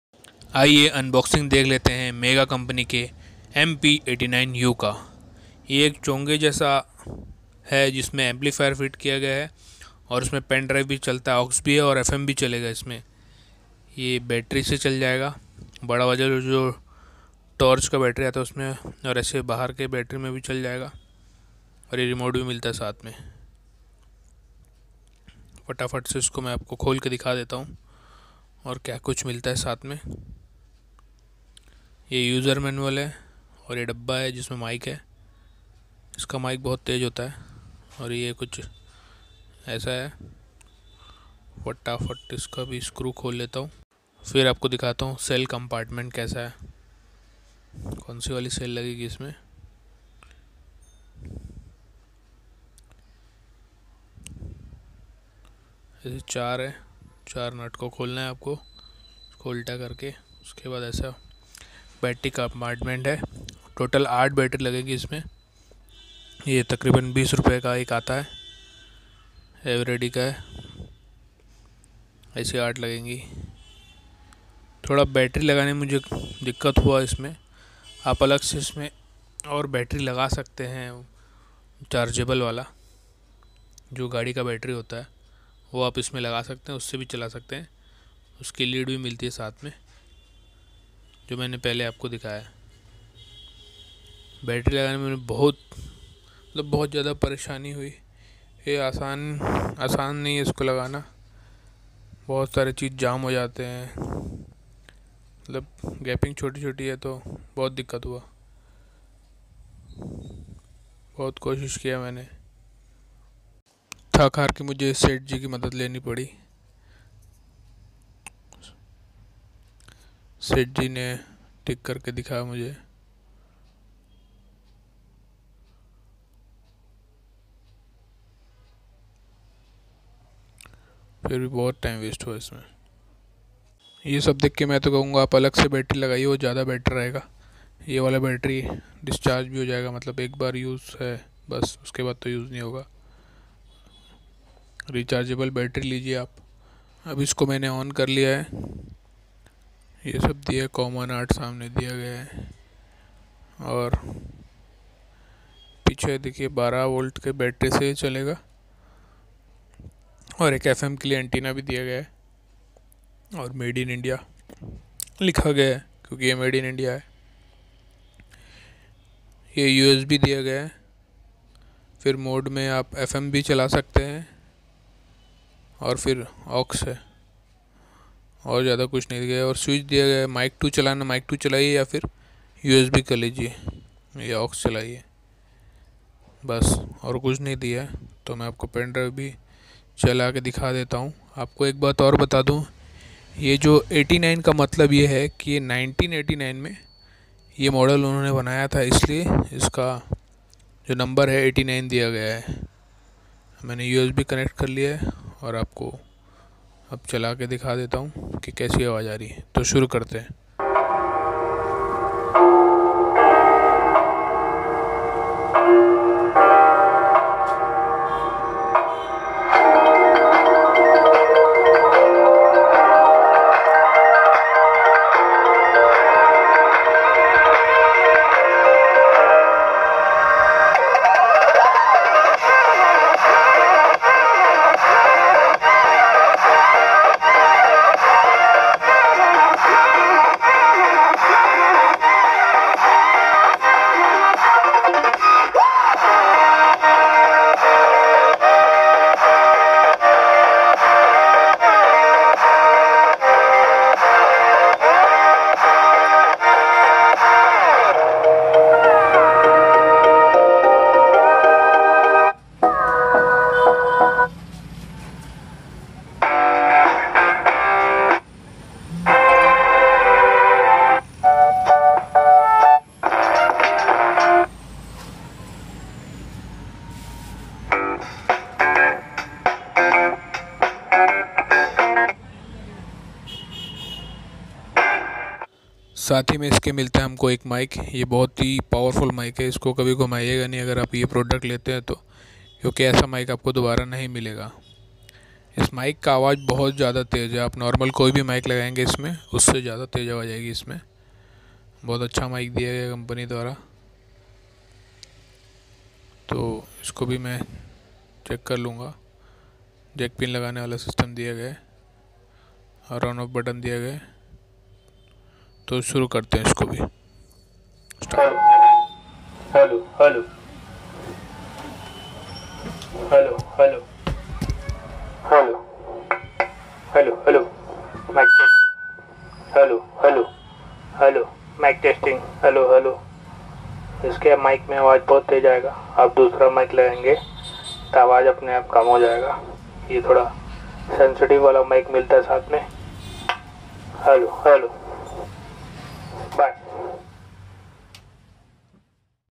MEGA MP 89U PORTABLE PA SYSTEM UNBOXING & REVIEW
Description

20W DC 12 V ,2MIKE,1 AUX

1. Built in 8 Ohms horn speaker.
2. Dual power source amplifier 6V DC (car battery) Or 7.5 V DC (5*1.5 v UM -1 dry cells.
3. Tone control.
4. Provision to connect an external 8 Ohms speaker.
5. Rugged Unit finished with weather resistant paint.
6. Ideal for mobile applications field publicity, controlling chaos etc.

Specification

Model
MP 89
Dimension
Power Requirements
12 V DC (car battery DC/8x UM-1 cell)
Power Output
20 watt max music power
Input Channels
2 Mikes | 1 Aux USB / SD Card player
Microphone
Unidirectional dynamic
Siren
Voice range
300 mtr (700 mtr in quiet area)

CREDITS: SANAT DEWANGAN JI